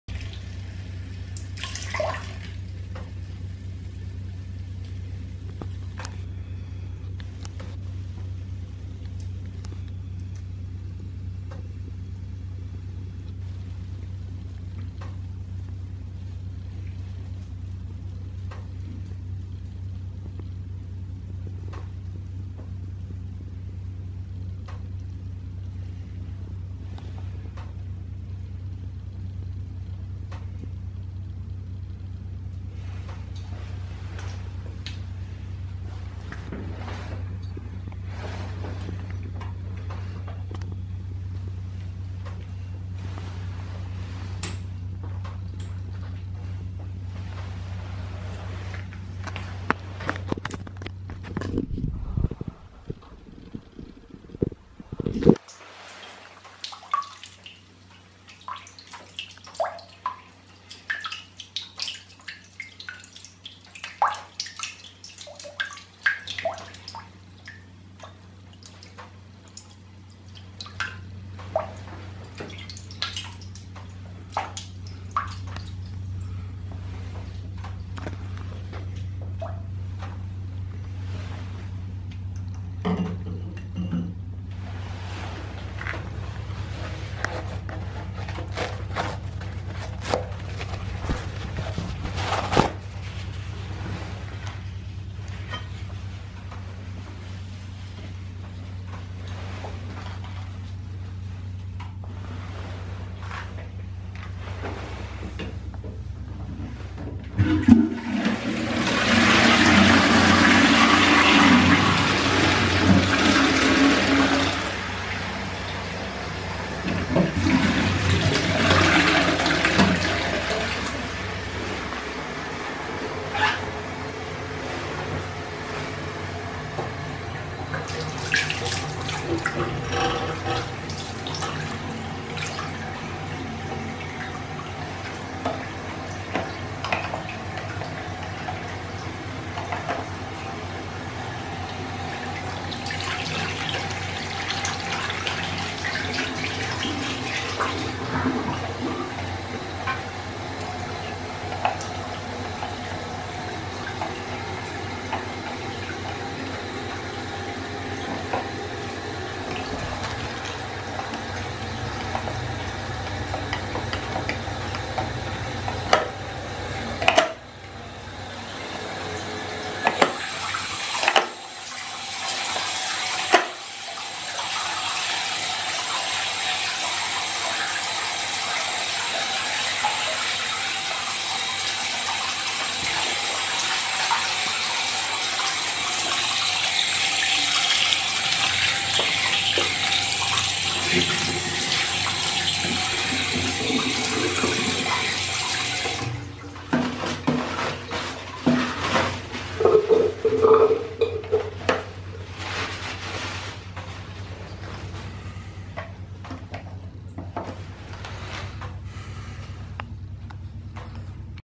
MVI 0191 toilet @marconi soap waste & + ...
trying around urinating, cleaning the toilet, washing hands, wasting soap